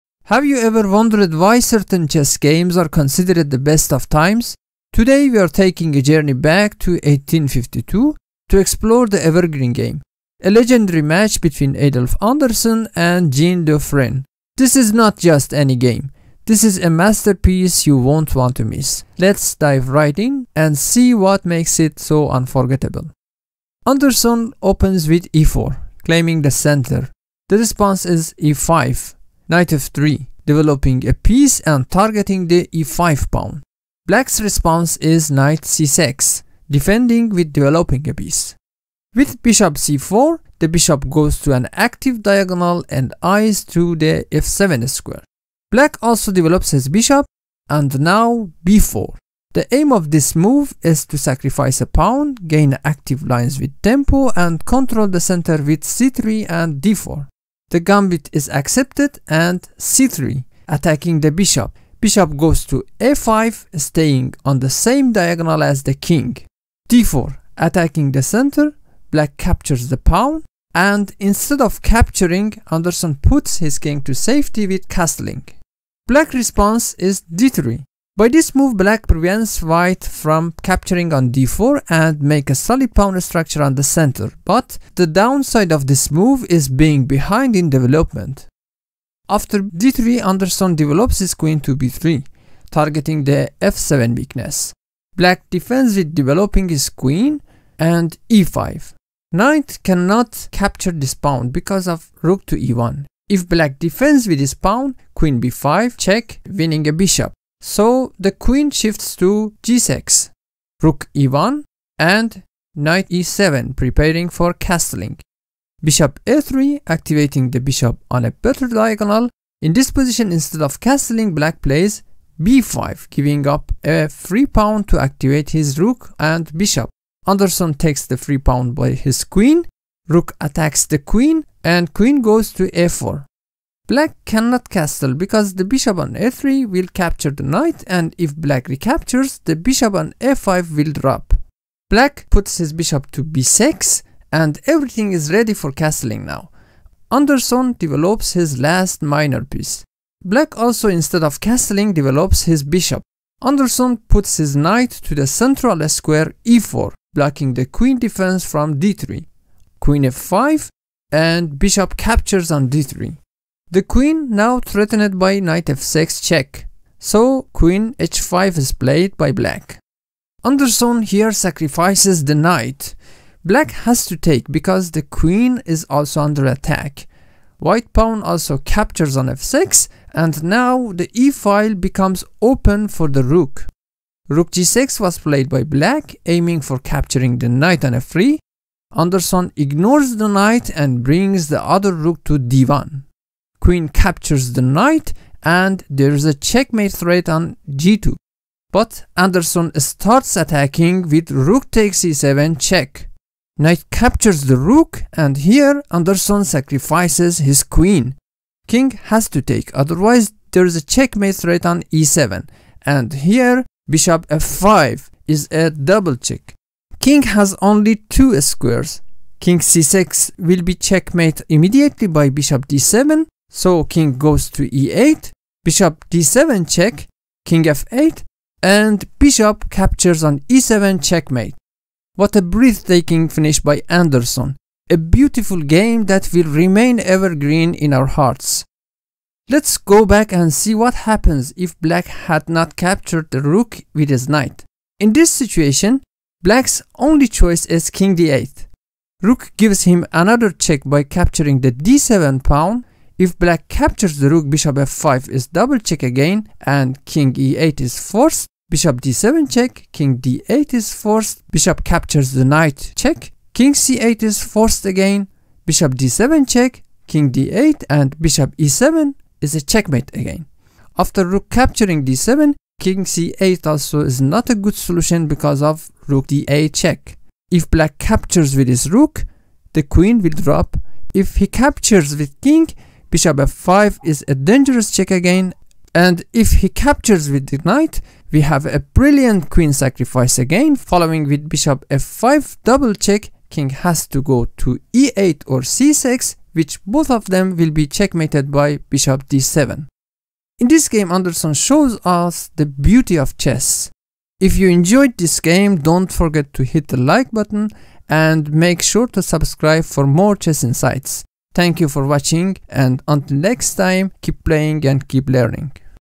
The Beauty of Chess: Anderssen's Evergreen Game
Dive into the realm of classical chess with "The Beauty of Chess: Anderssen's Evergreen Game." In this video, we take an in-depth look at one of the most celebrated games in chess history, played between Adolf Anderssen and Jean Dufresne in 1852. Known for its dramatic and artful checkmate, this game showcases strategic brilliance and timeless tactics. Whether you're a seasoned chess player or just starting out, this analysis will enrich your understanding of chess strategy and history. Join us as we explore the moves that made this game an evergreen masterpiece in the chess world.
Every contribution is cherished. Thank you! 🙏☕️♟️